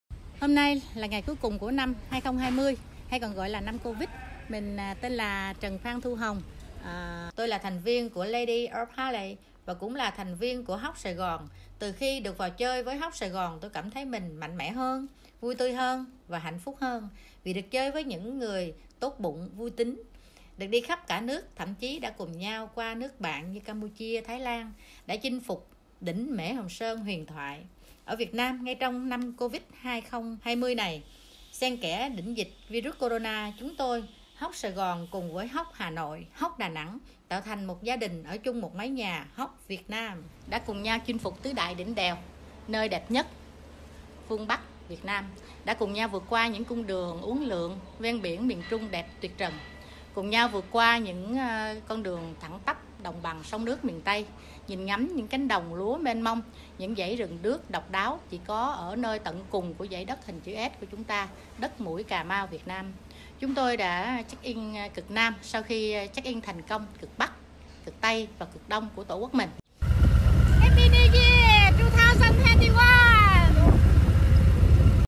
H.O.G. Vietnam Shoutout | Harley-Davidson
Shoutout from the Vietnam Harley Owners Group, represented by Hong Tran Phan Thu, for the 1st H.O.G. Virtual Rally.